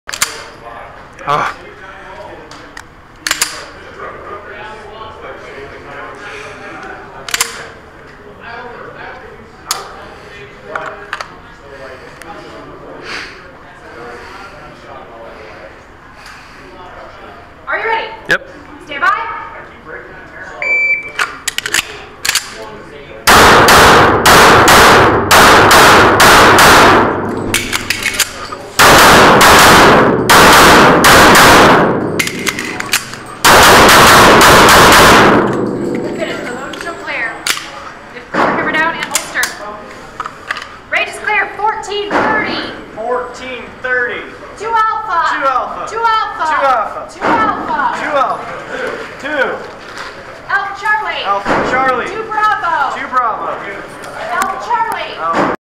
2 15 17 AIPS @ Crossroads - Stage 3 Unloaded Start
Driven by sheer distaste for SSTK shooters, Scott put together an 8, 8, 6 sequence with an UNLOADED start.